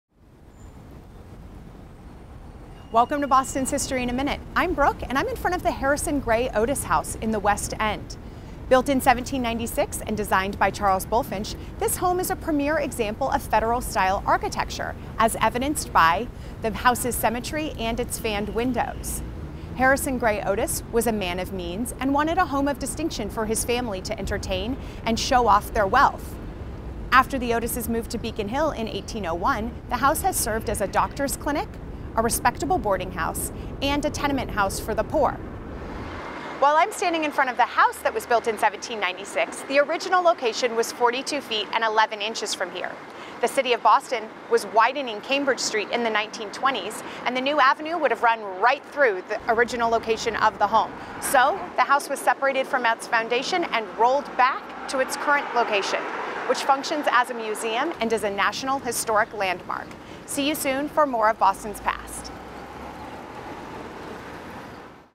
Boston History in a Minute: Harrison Gray Otis House
You have probably walked by this house in the West End and not known that it is over two hundred years old and an important piece of Federalist architecture. And while it's very, very close to its original location, it has been moved--find out why in 1 minute.